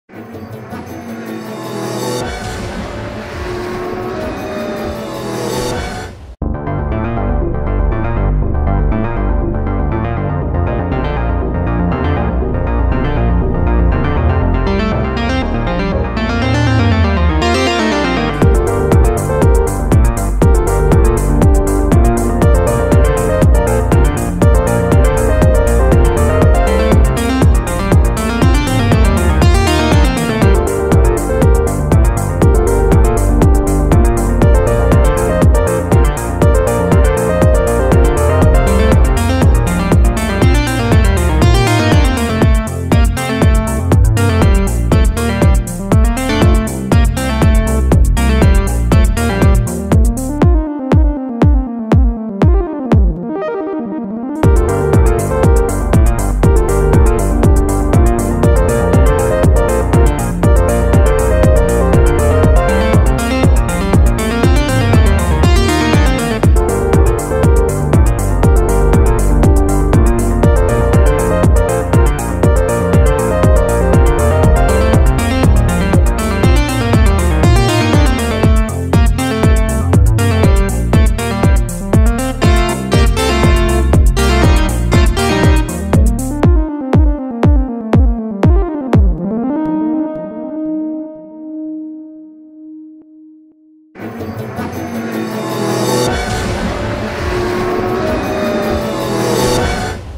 2019 Hyundai Santa Fe 1st. look on the streets and roads of South Korea
2019 Hyundai Santa Fe 1st. look on the streets and roads of South Korea